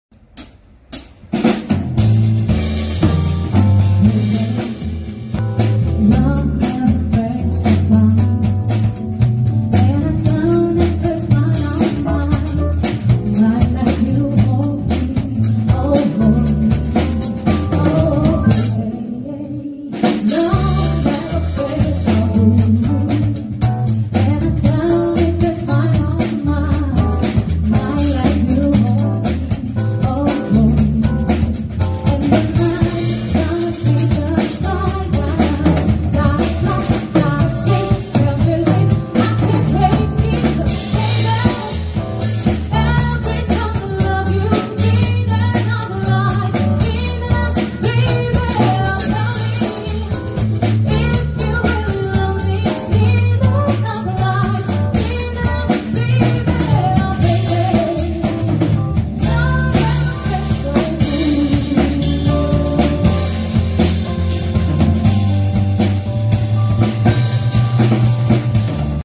와이준밴드 비대면공연 촬영현장!!
오랜만에 와이준밴드 공연촬영이 있었어서 강원감영에 다녀왔습니다

얼른 코로나가 없어져서 편하게 공연하는 날이 오기를!!